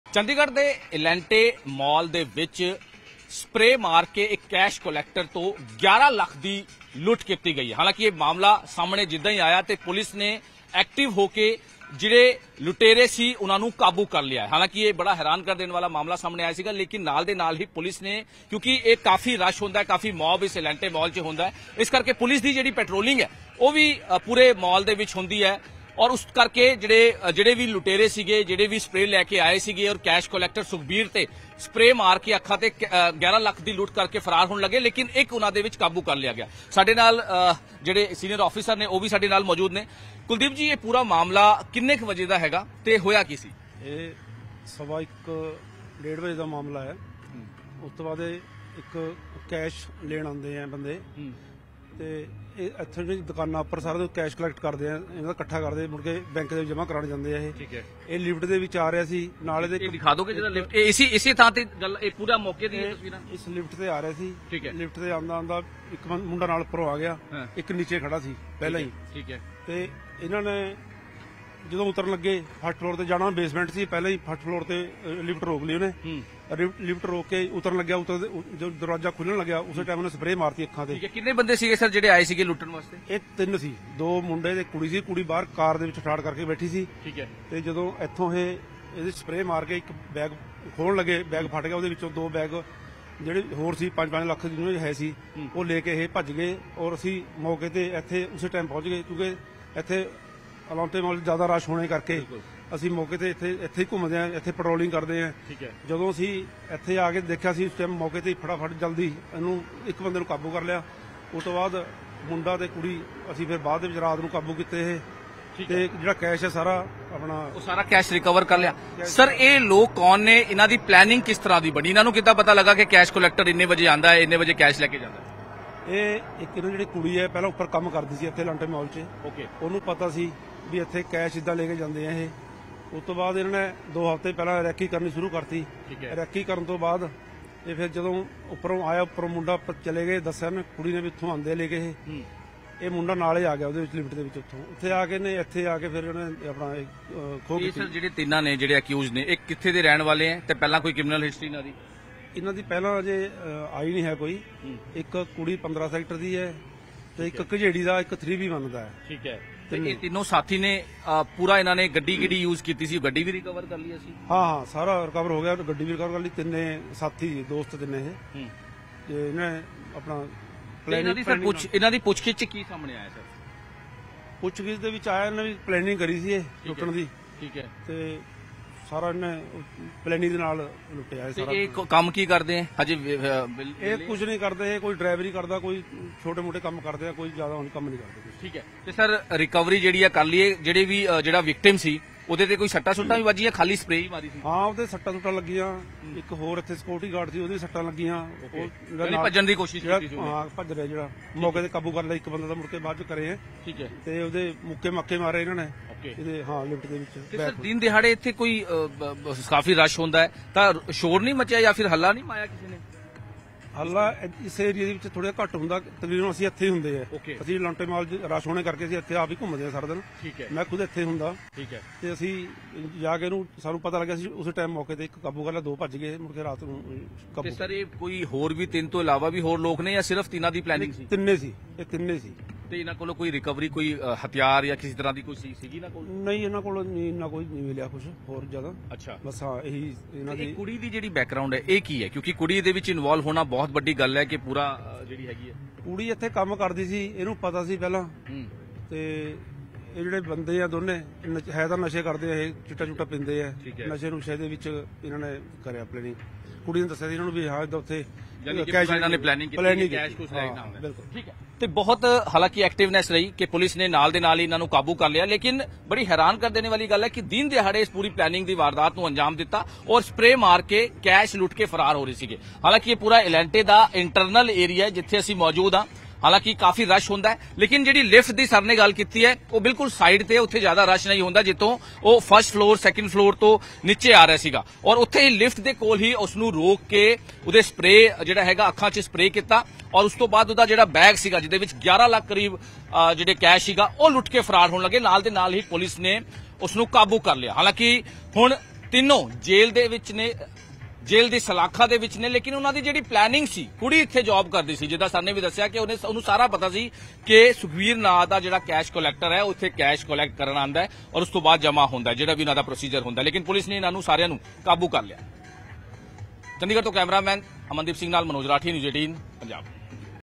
Chandigarh Elante Mall Loot | ਇਲਾਂਟੇ ਮਾਲ 'ਚ ਆਹ ਕੀ ! | Chandigarh News | Punjab News | N18V
ਚੰਡੀਗੜ ਦੇ ਇਲਾਂਟੇ ਮਾਲ 'ਚ 11 ਲੱਖ ਦੀ ਲੁੱਟ। ਅੱਖਾਂ 'ਚ ਸਪ੍ਰੇਅ ਕਰ ਲੁੱਟ ਲਿਆ ਕੈਸ਼ੀਅਰ। ਮਾਲ ਦੀ ਲਿਫਟ 'ਚ ਦਿੱਤਾ ਵਾਰਦਾਤ ਨੂੰ ਅੰਜਾਮ। ਮਾਲ 'ਚ ਕੰਮ ਕਰ ਚੁੱਕੀ ਲੜਕੀ ਨੇ ਦਿੱਤਾ ਸਾਥ। ਸਾਥੀਆਂ ਨਾਲ ਮਿਲ ਕੇ ਬਣਾਇਆ ਸੀ ਪਲਾਨ। ਪੁਲਿਸ ਨੇ ਤਿੰਨਾਂ ਲੁਟੇਰਿਆਂ ਨੂੰ ਕੀਤਾ ਗ੍ਰਿਫਤਾਰ।